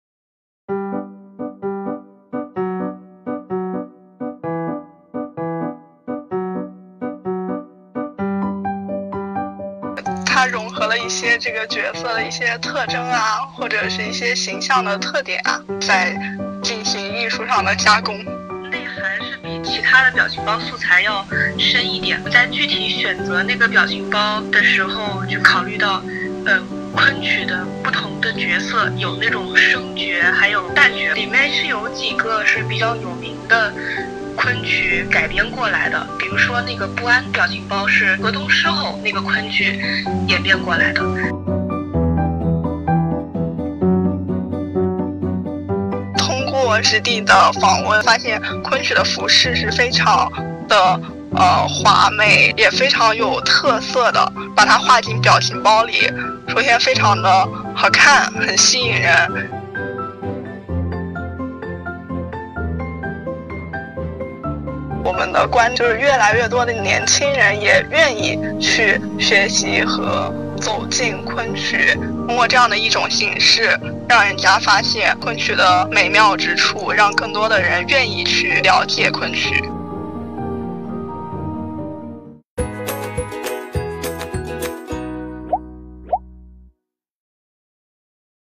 Students created a set of emoticons based on Kunqu opera.
Students from Nanjing University of Aeronautics and Astronautics have created a set of emoticons based on intangible cultural heritage Kunqu opera, hoping to make more young people love Kunqu opera in this way.